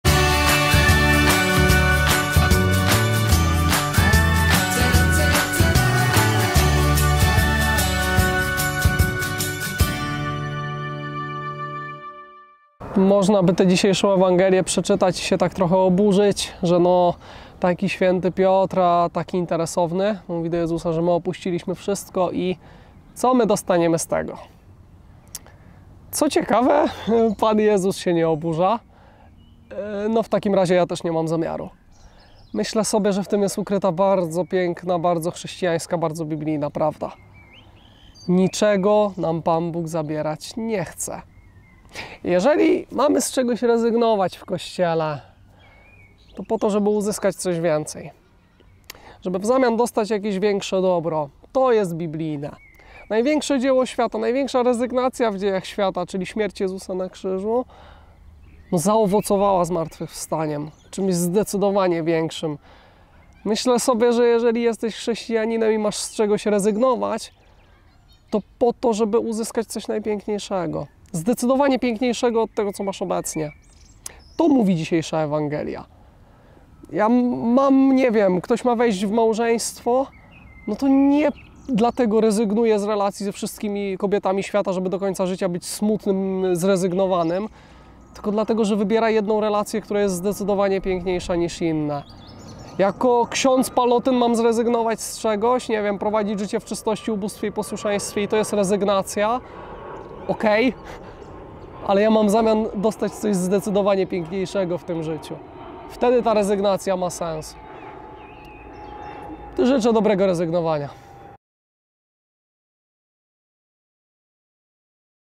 Pallotyński komentarz – 30 maja 2023 - kl. Paweł Strojewski SAC
Niczego Pan Bóg zabierać nam nie chce! Kl. Paweł Strojewski SAC zaprasza na pallotyński komentarz.

Syr 35,1-12
Mk 10,28-31